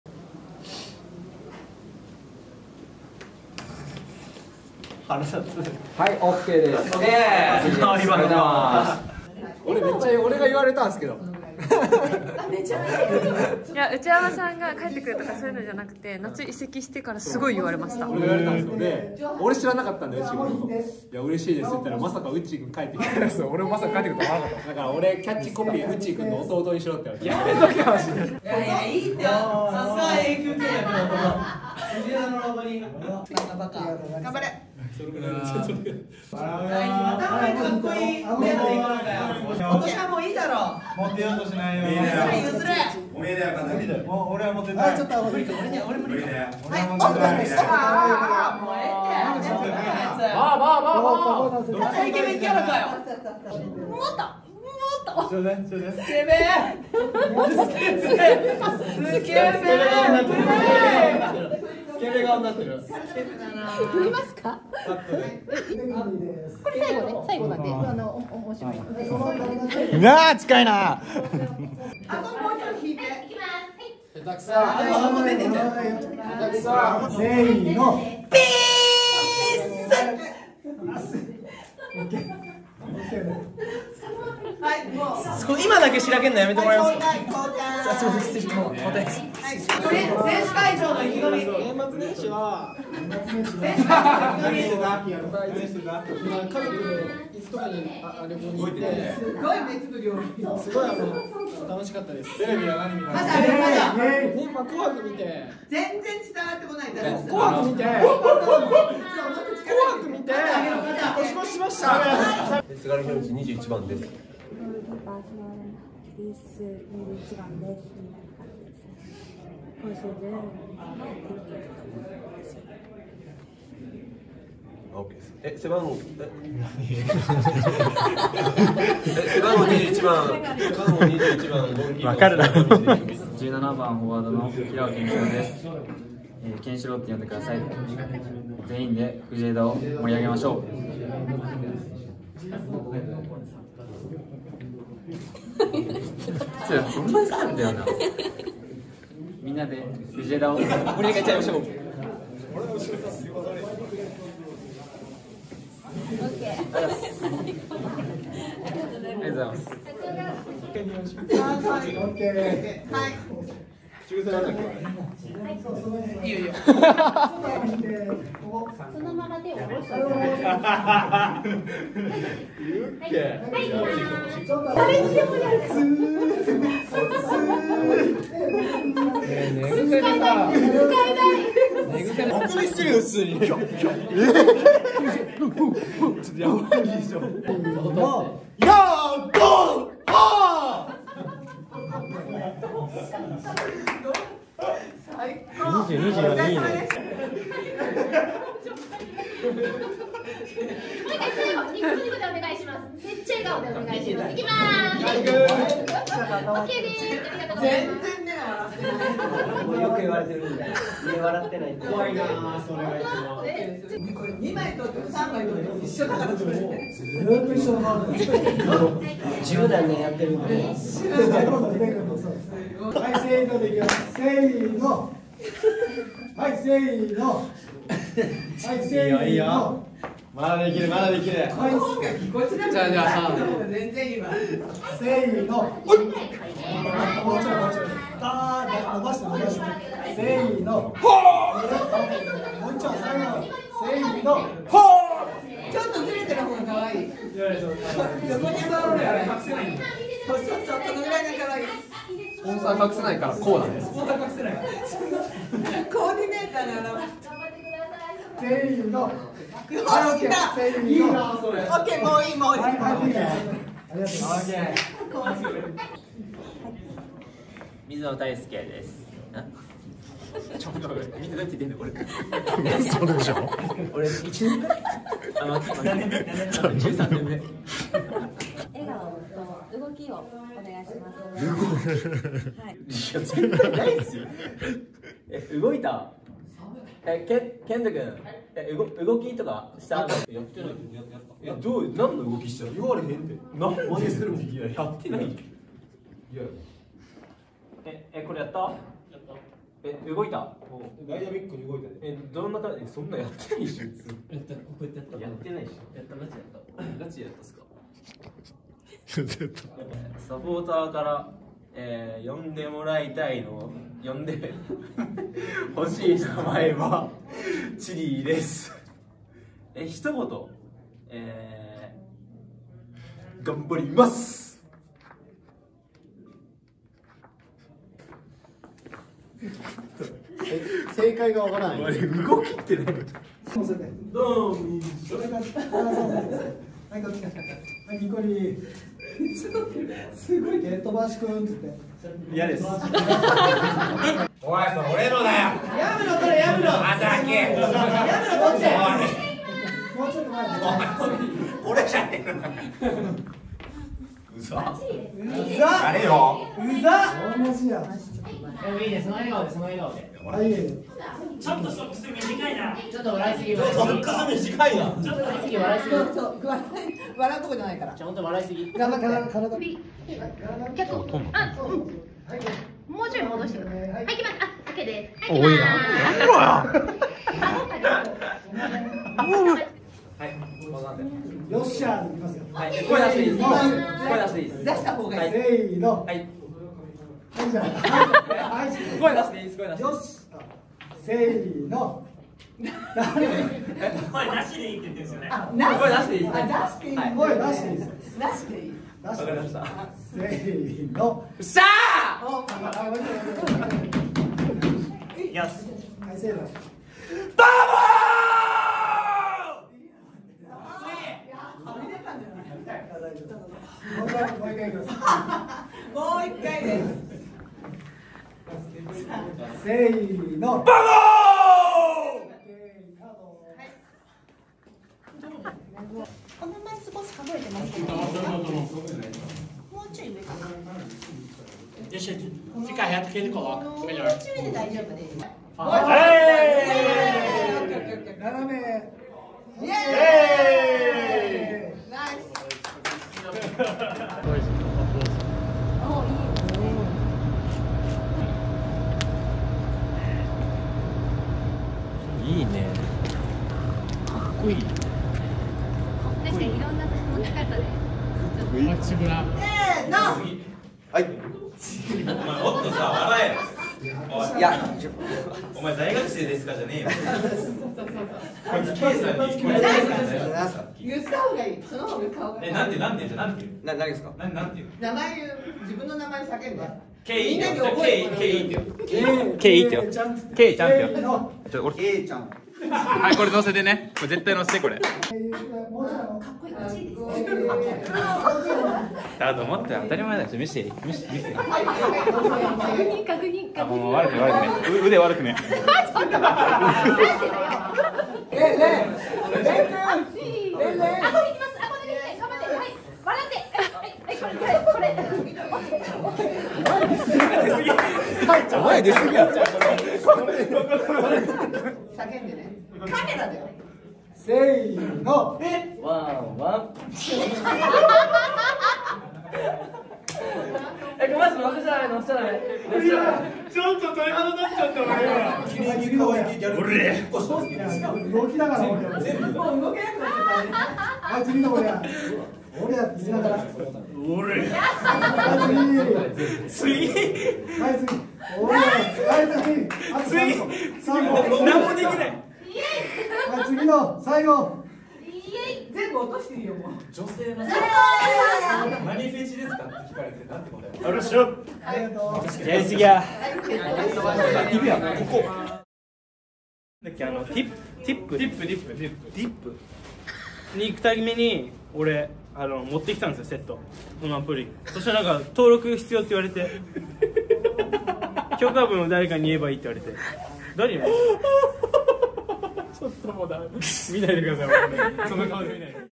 【藤枝MYFC】2024シーズン 写真撮影の裏側公開！
1月8日（月・祝）、2024シーズンの新チームが始動した日に、写真撮影も一緒に行っていました！
その裏側を大公開！選手同士のやりとりに注目です！